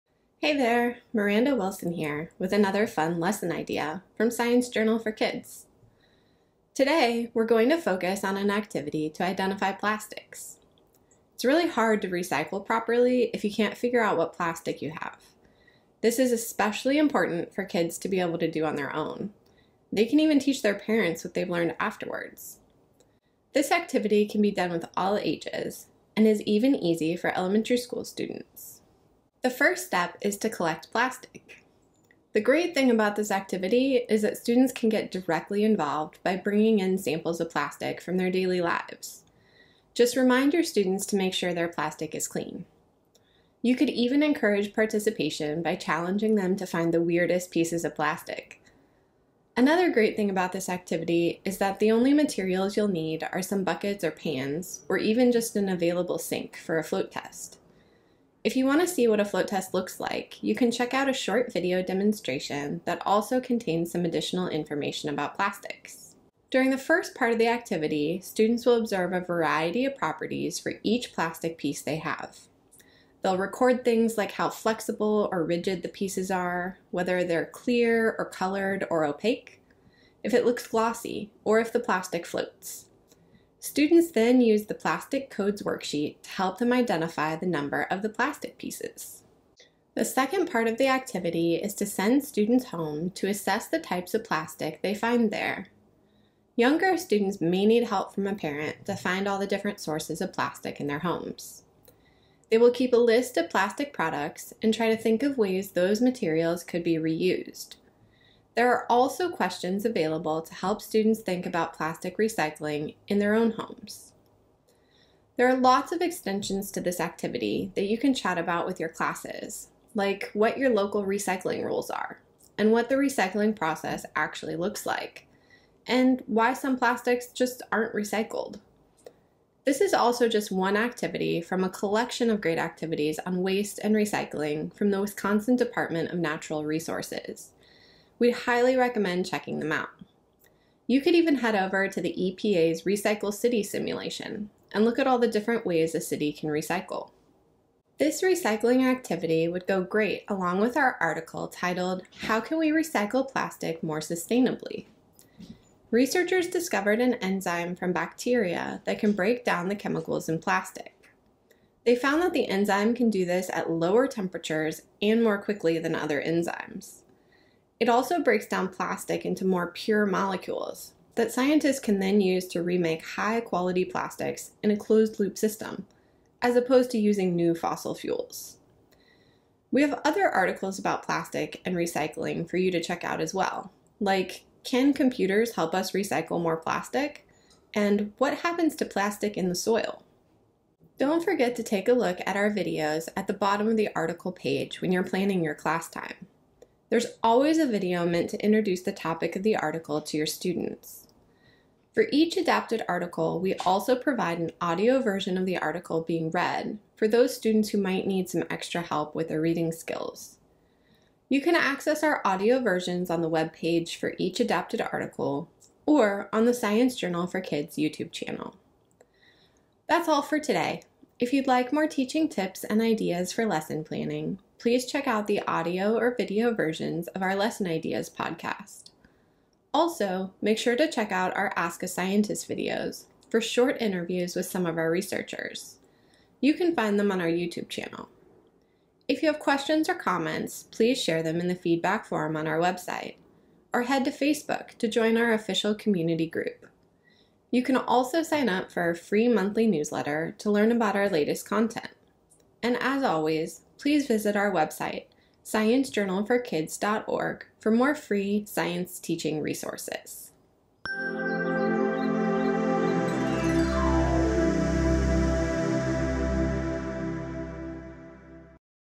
Recycling plastic: hands-on activity + readings for science class, teaching tips, lesson ideas
Check out this quick tutorial video of our newest adapted articles from Science Journal for Kids. Our goal is to highlight and explain some of the additional resources we’ve curated to help with lesson planning.